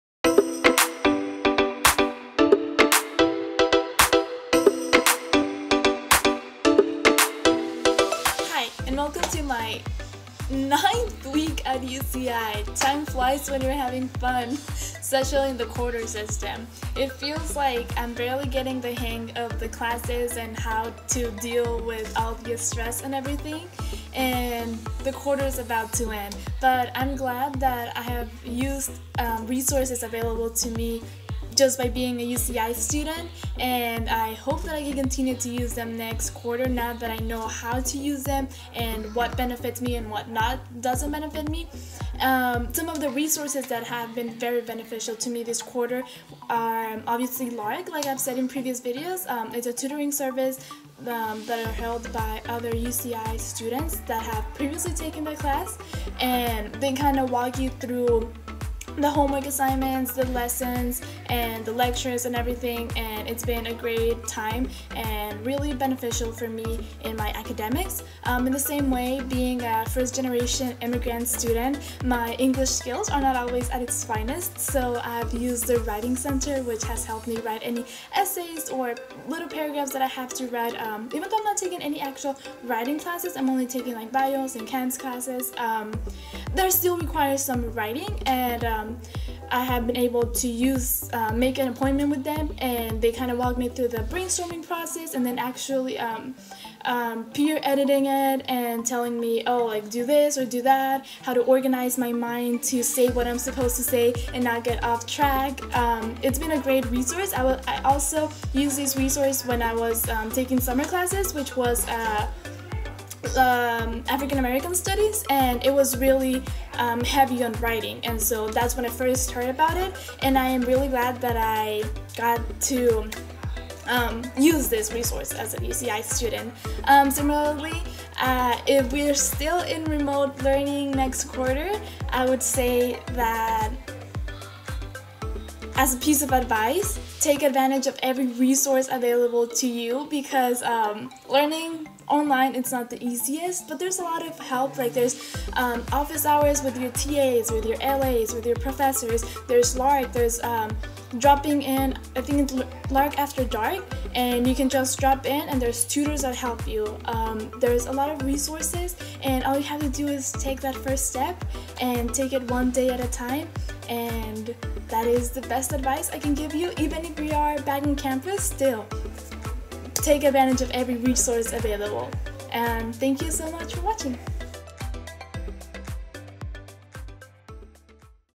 What UCI Resources Have You Been Using? What Advice Do You Have For Incoming Students?: Gretta Ozuna
In Gretta’s Week 9 First Things First update, she talks about using UCI resources like LARC tutoring and writing consultations at the Center for Excellence in Writing and Communication. While learning online is difficult, Gretta says her best advice for incoming students is to learn about and leverage every resource available.